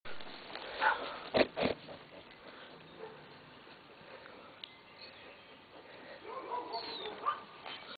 Baby Goats
My two day old baby goats hanging out in the barn